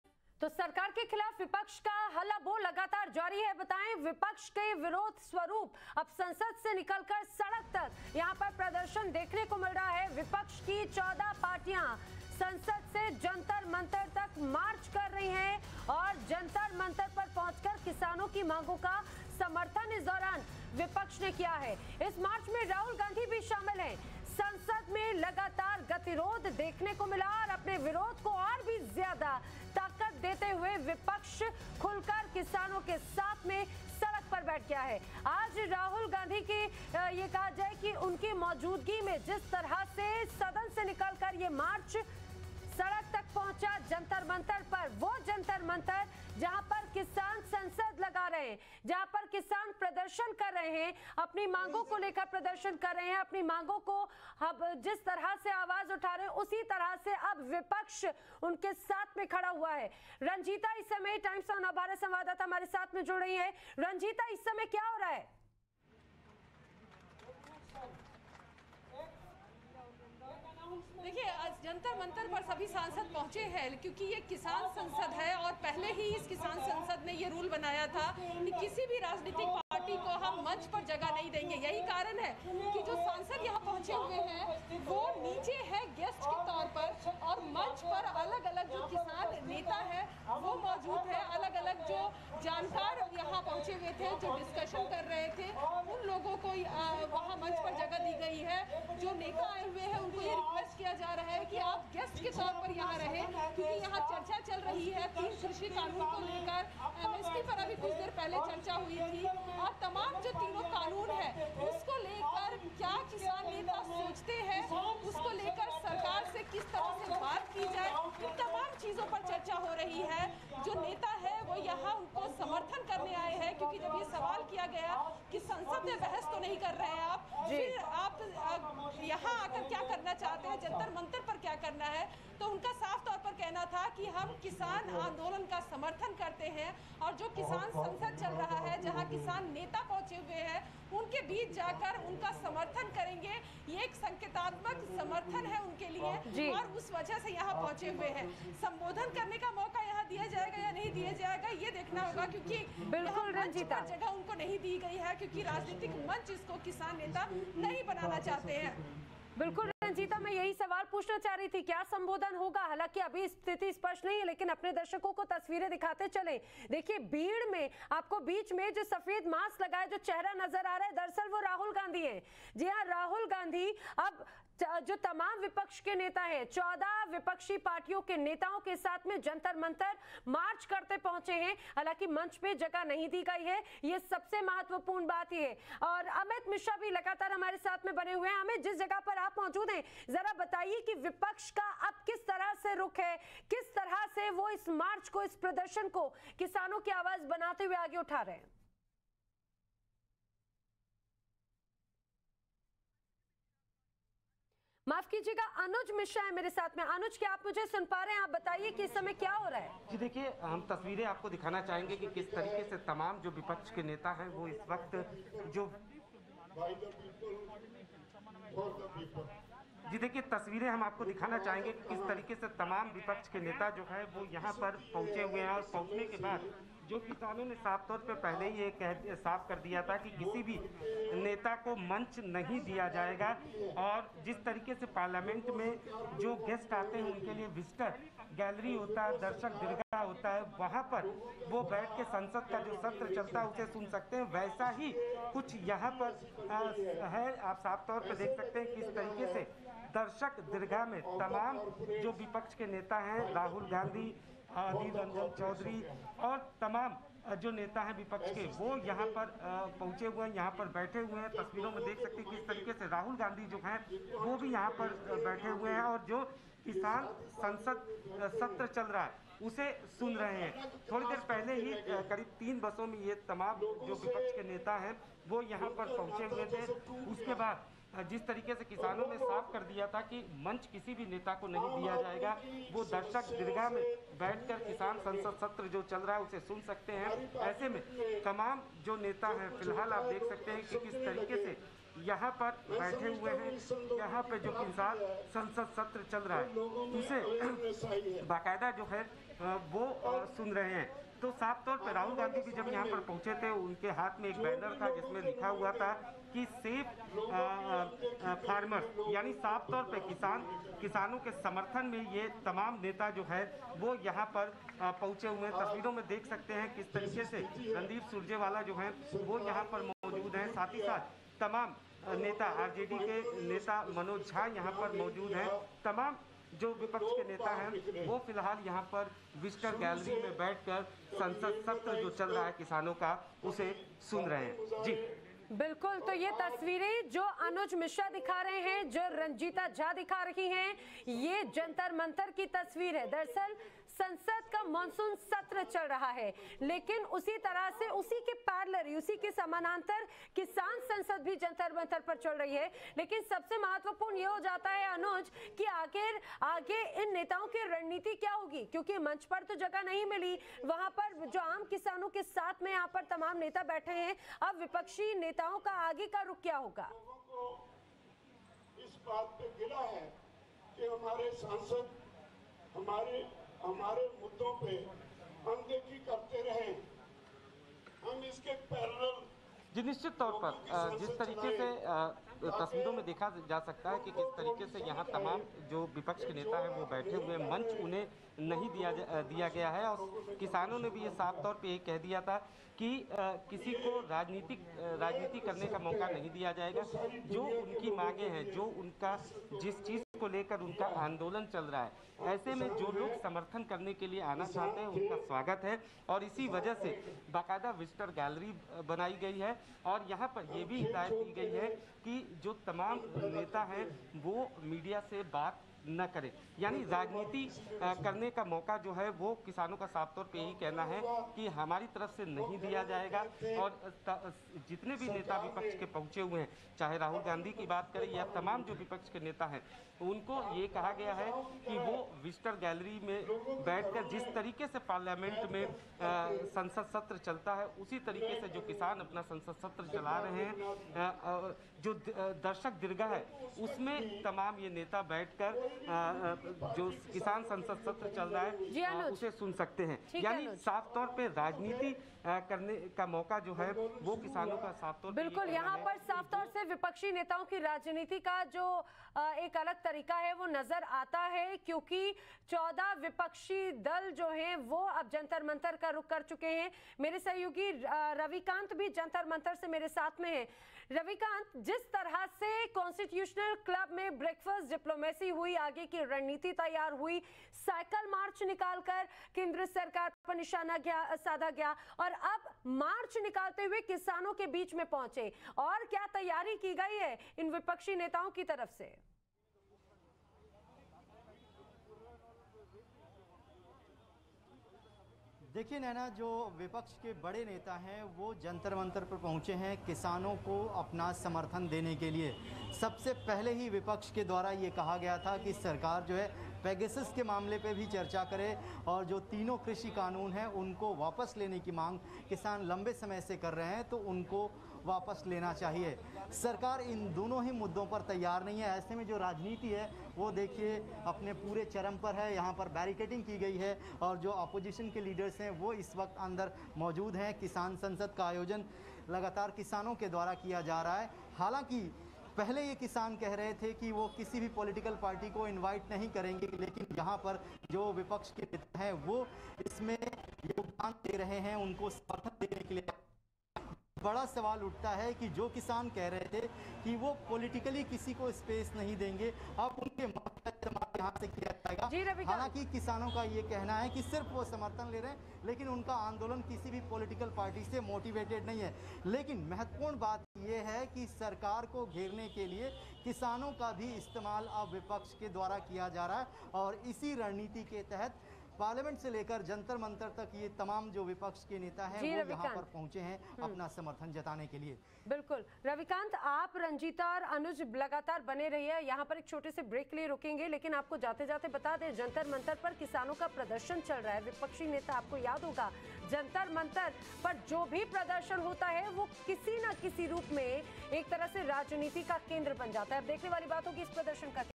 किसानों के साथ कृषि कानून के विरोध में किसान संसद में पहुंचे Rahul Gandhi, मंच से नीचे खड़े आए नज़र
Rahul Gandhi आज कृषि कानून के विरोध में किसानों के साथ जंतर मंतर पहुंचे | वहां उन्होंने Modi सरकार की तरफ से लागू Farm Bills का विरोध दर्ज कराया |

Airtel - 327
Hathway - 321
DEN - 899